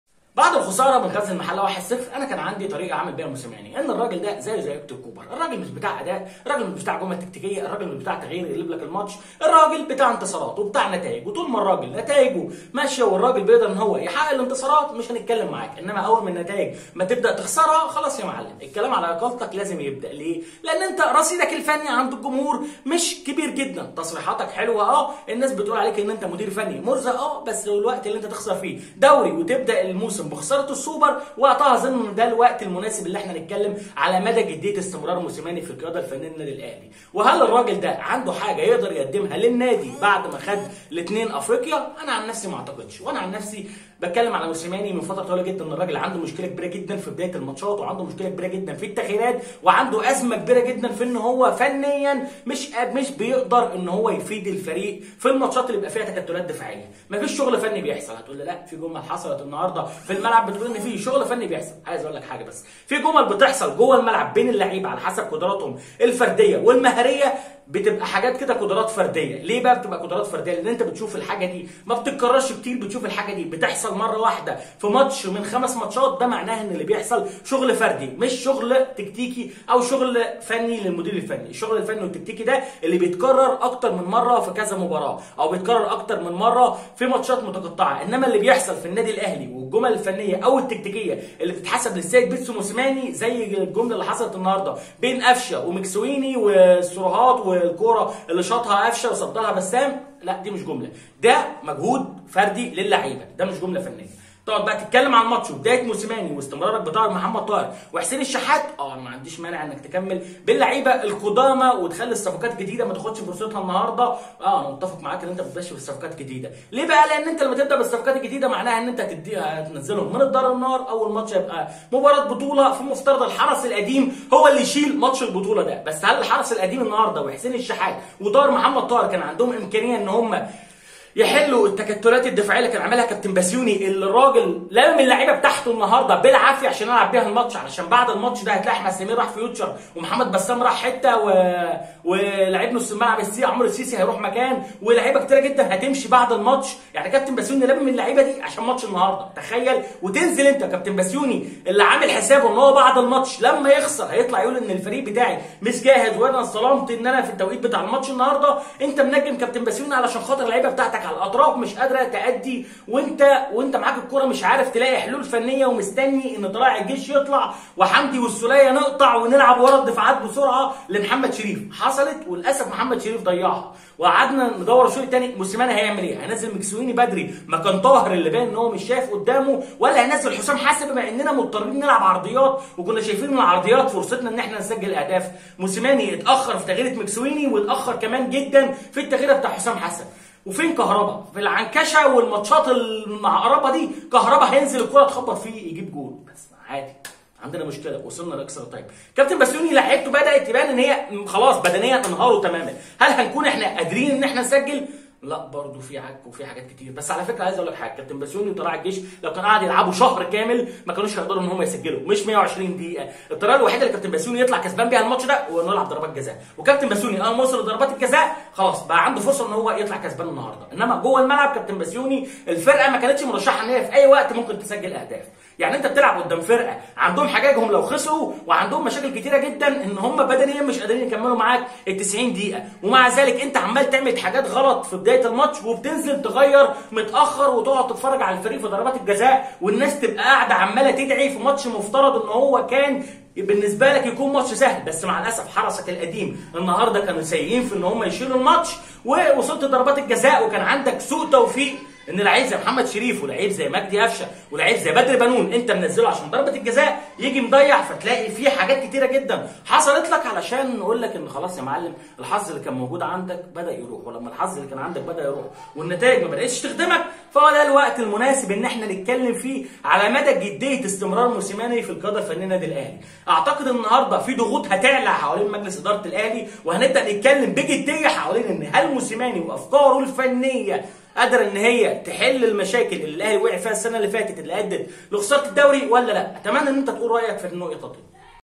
ملخص وتحليل مباراه الاهلي وطلائع الجيش هل حان الوقت للحديث عن اقاله موسيماني
تحليل مباراه الاهلي-طلائع_الجيش ضربات_جزاء_الاهلي_طلائع_الجيش اليوم في كاس السوبر المصري ملخص مباراه الاهلي اليوم اهداف مباراه الاهلي ملخص مباراه الاهلي والجيش